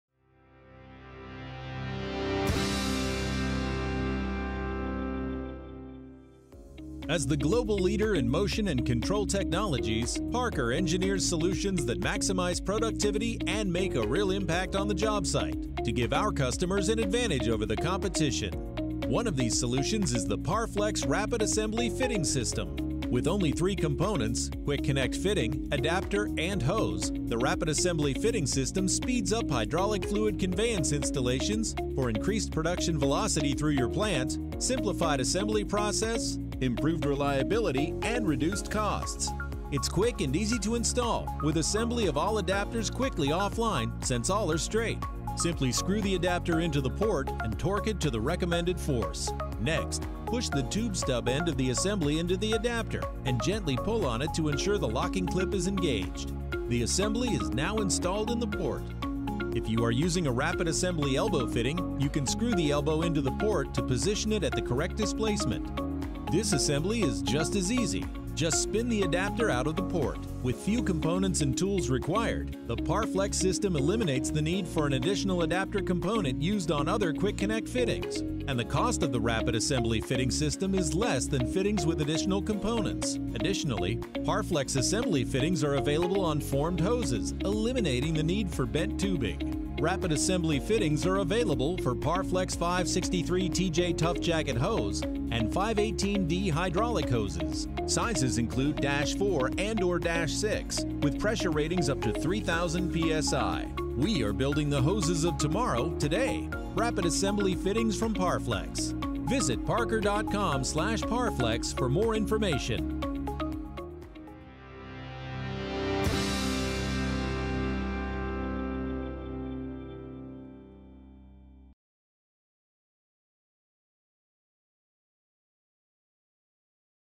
How to Assemble the Parker Parflex 56 Series Rapid Assembly Fittings
How to video showing how to assemble Parkers 56 Rapid Assembly Fittings. These fittings make installs quick, easy and speed up installation on Parflex 100R7 and 100R17 hoses.

Parker's Rapid Assembly Fittings are designed to be used with certain Parflex hydraulic and pneumatic hoses. Their quick connect design offers plug and play convenience in a very compact, economical system. The connector design is durable and easy to install, eliminating the need for wrenches and other tools which allows hoses the advantage of easy installation and improved assembly time. With one adapter, customers speed up assembly installations by 50%. Simply screw in the adapter and push the tube stub fitting into the adapter.

Use with Parflex hoses qualified for 56 Series fittings.

Markets:
• Construction
• Agriculture
• Turf Care

Features/Benefits:
• Compact for improved assembly time
• Light weight for reduced system weight
• Quick connecting fittings eliminate the need for additional tools

Applications:
• Hydraulic and pneumatic systems where plug and play convenience is needed
• Pilot Lines
• Joystick
• Skid Steer Chassis